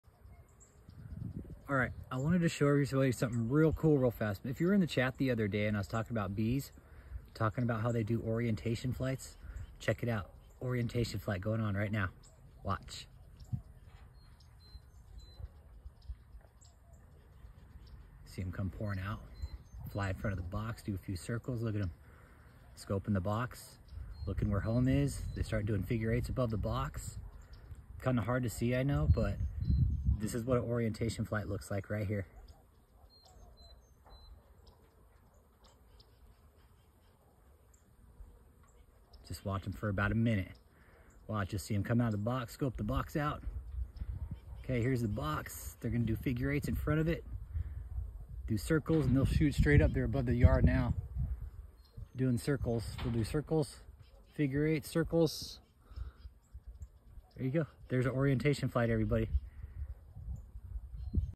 Africanized Bees First Orientation Flight
Here is what an orientation flight looks like for bees when they first emerge from their box. Notice the figure 8’s they do in front of the box, this is a similar pattern that the sun takes when it sets and rises over the year.